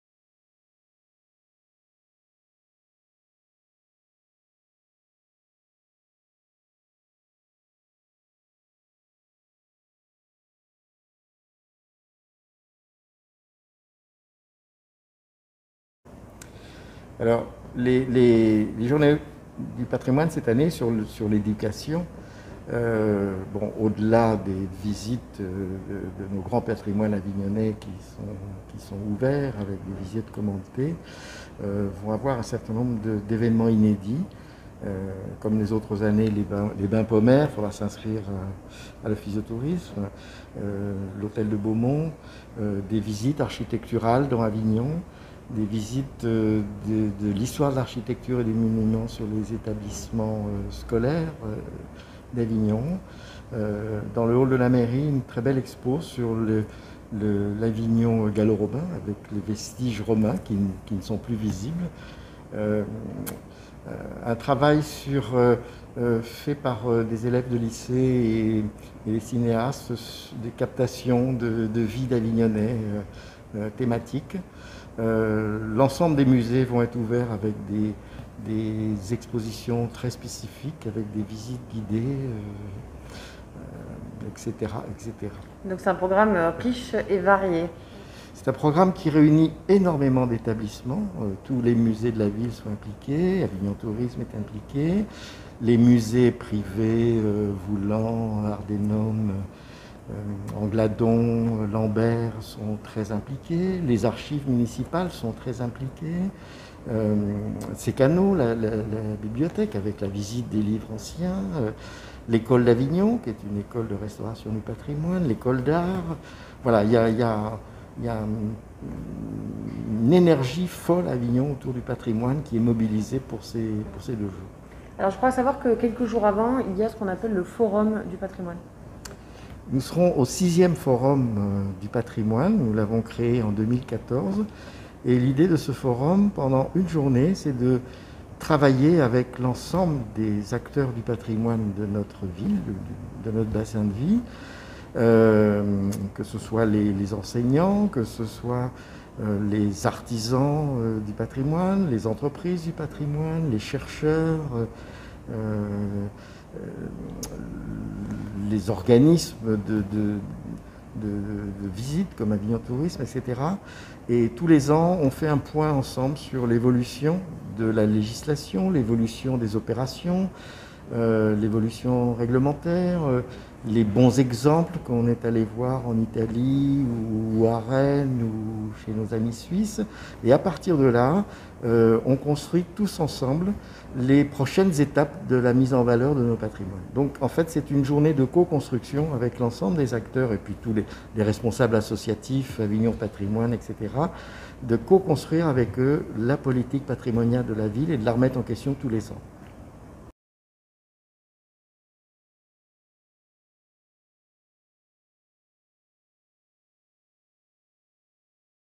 Les Journées Européennes du Patrimoine, interview de Sébastien giorgis, élu à la Ville d'Avignon
Les Journées Européennes du Patrimoine se tiendront les samedi 19 et dimanche 20 septembre !

Venez (re)découvrir les monuments d'exception qui font la renommée mondiale d'Avignon !